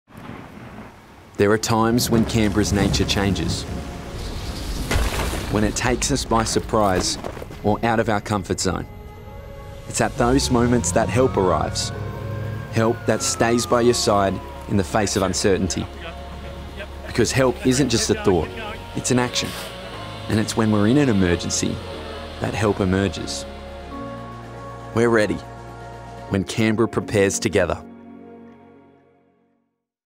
We’re ready when Canberra prepares together (ACTSES 30 sec TVC)
We’re ready when Canberra prepares together.

Our ACT State Emergency Service volunteers are here to help and keep our community safe. Help them by getting your home and family prepared for storm.

Complete or update your 20-minute Survival Plan and Be Storm Ready